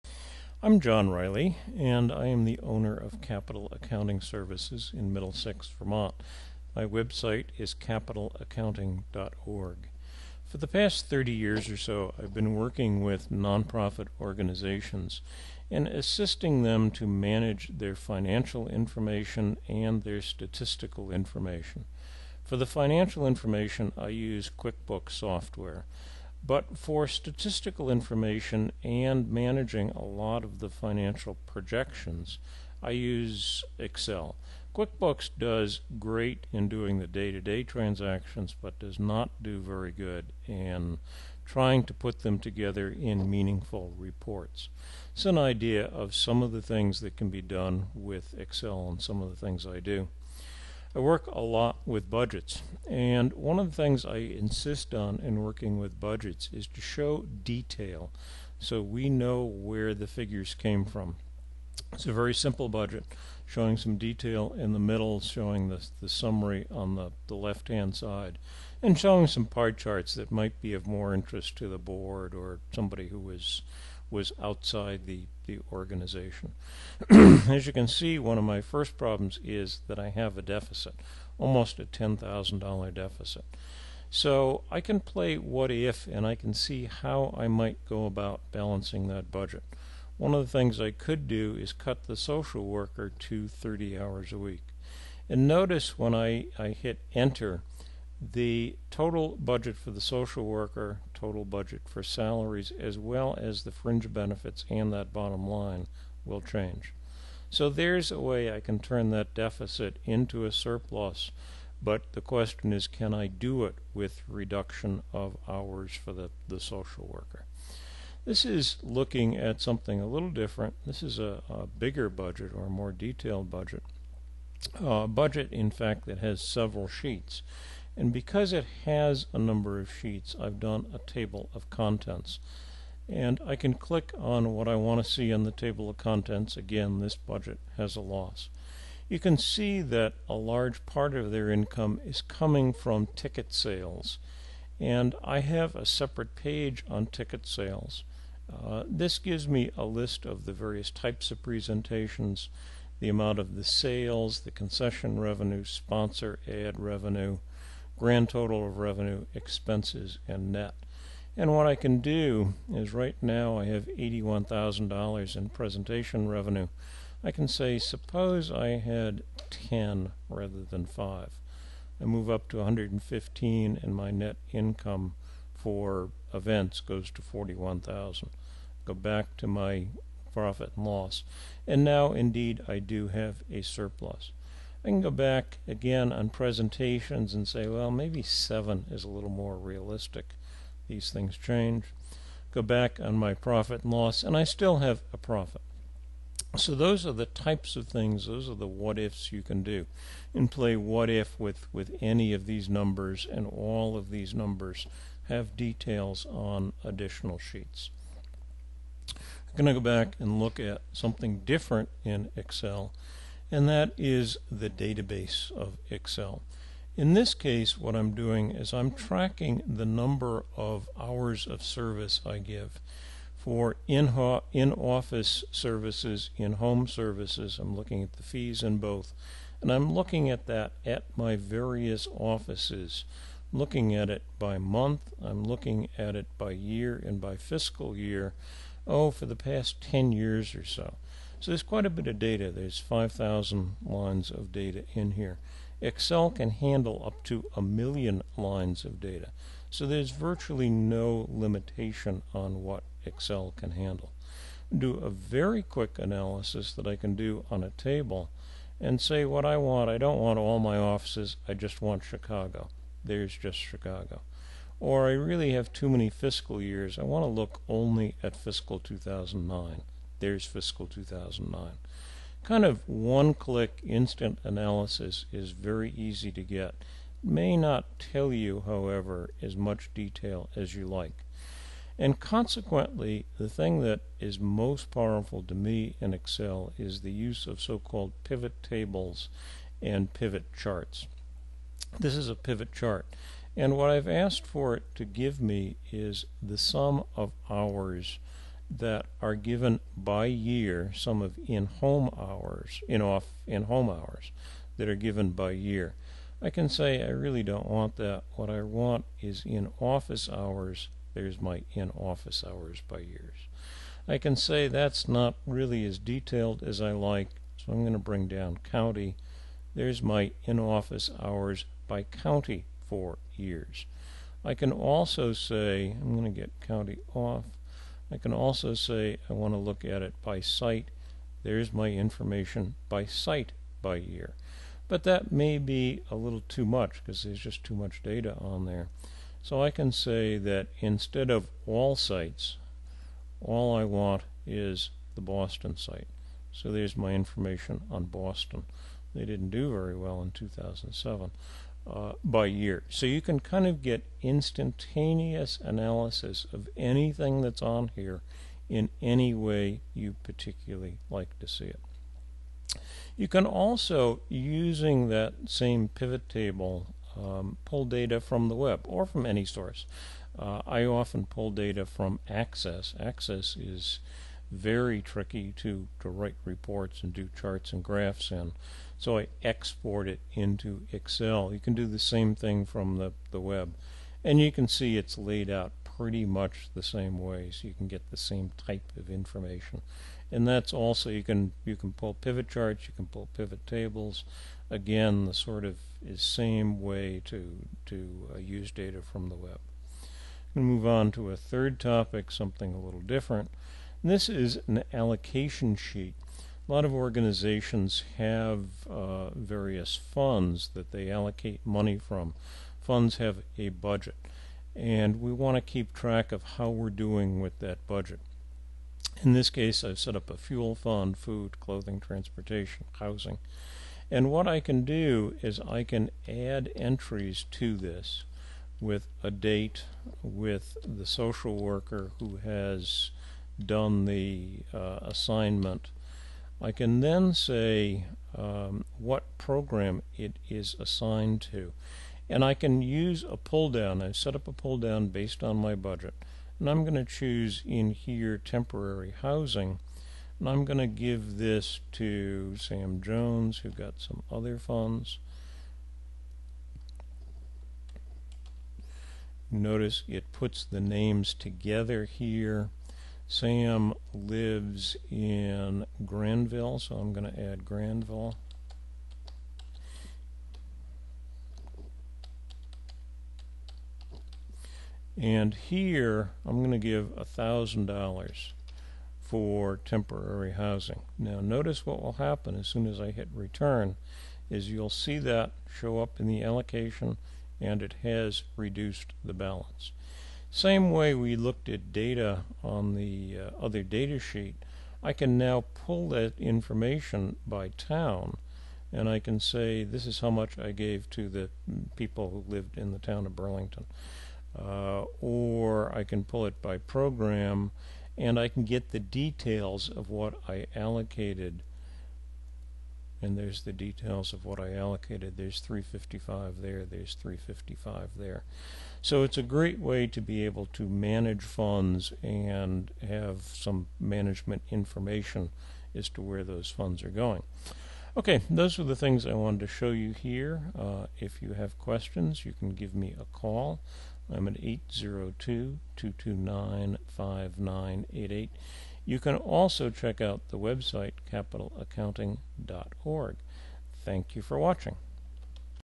Excel apps for non-profit organizations
This video is intended to demonstrate how excel can be used for budgeting, data analysis and fund allocation for non-profit organizations